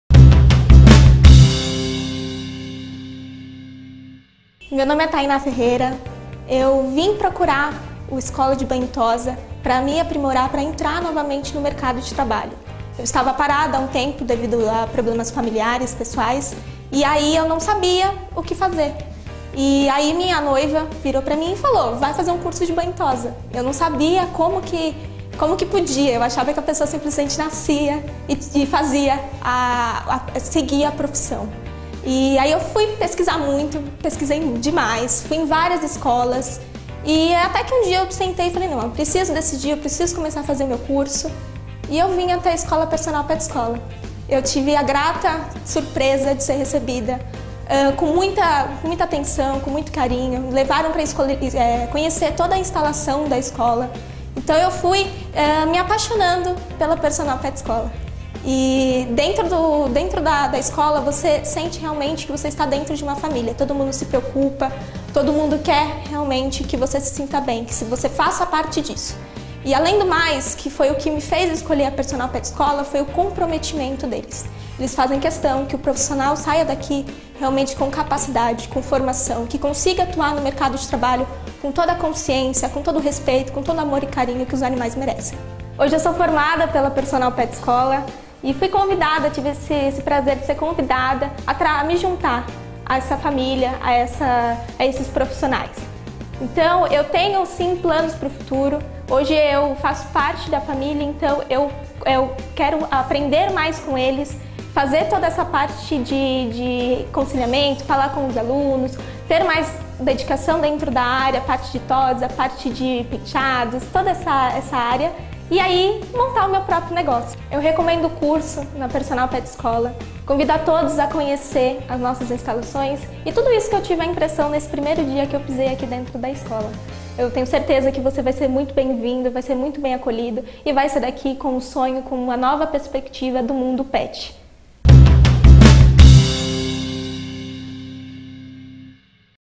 Personal Pet Escola - Depoimento de alunos
Nossa aluna Tayna e hoje colaboradora conta um pouco sobre sua trajetória até encontrar a Personal Pet Escola e suas expectativas para o futuro.